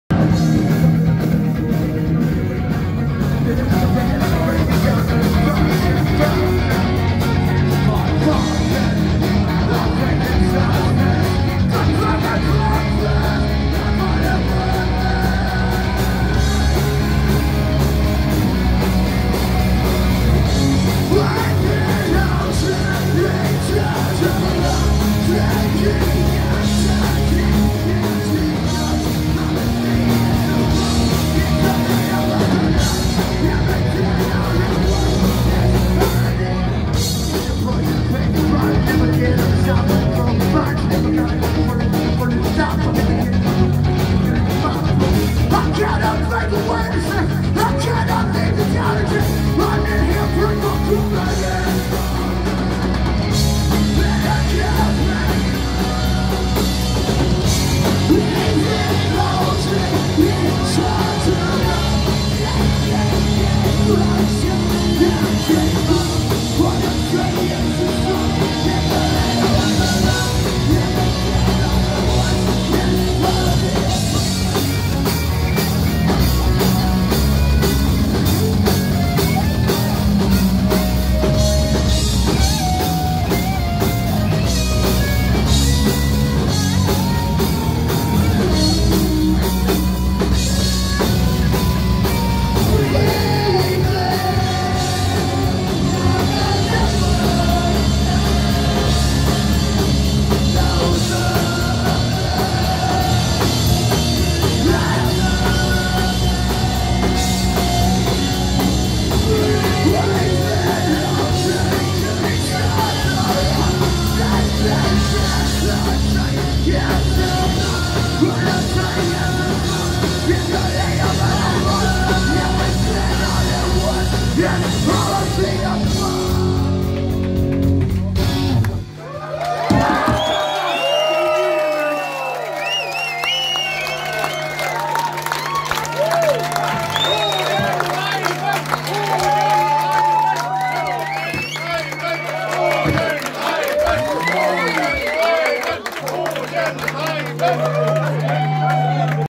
Get Dead live in Hamburg [04/2019]
Great show on a Friday night straight in the heart of St. Pauli ⚓❤☠